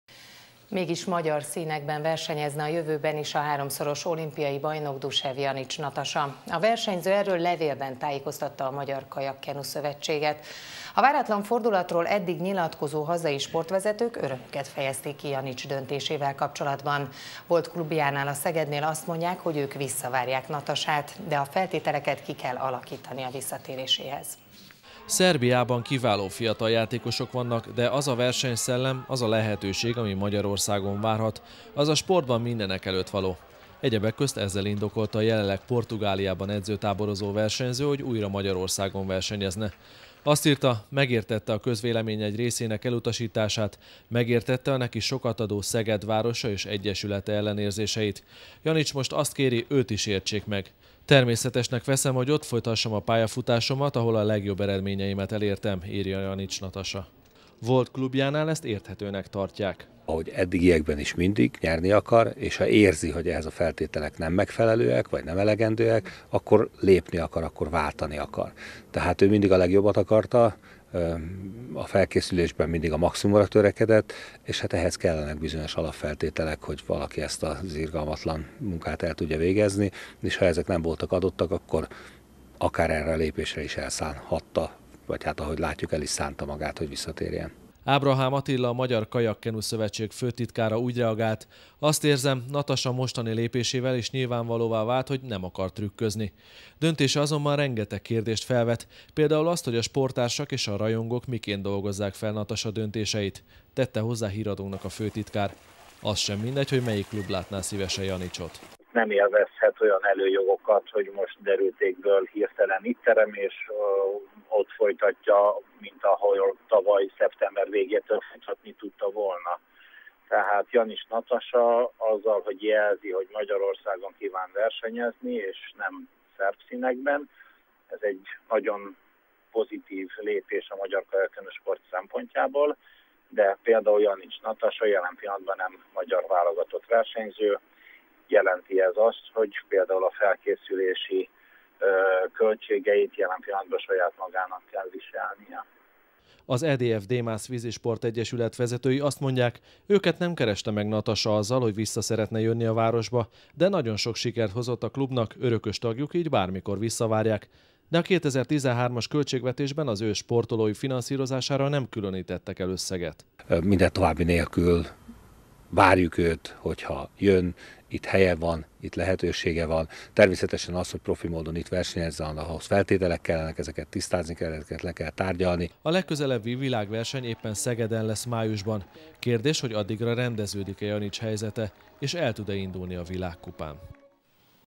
Janics Natasa mégis magyar maradna
Mégis magyar színekben versenyezne a jövőben is a háromszoros olimpiai bajnok Dusev-Janics Natasa.